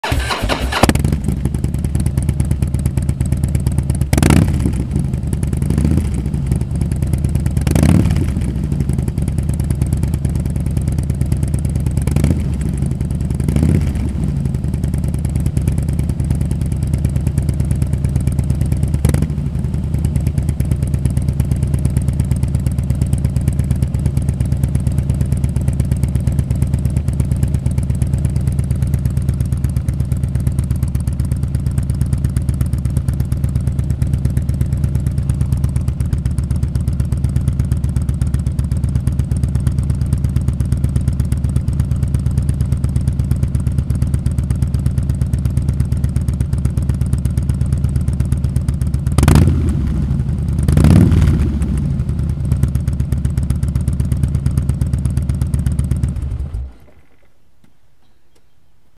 Cobra 3-inch Slip-on mufflers for HDs.mov
You can now listen to the new Cobra 3-Inch slip-on mufflers. Hear the bike start up, rev a few times and idle. Notice the deep throaty sound out of this $299.95 set of high quality slip-ons.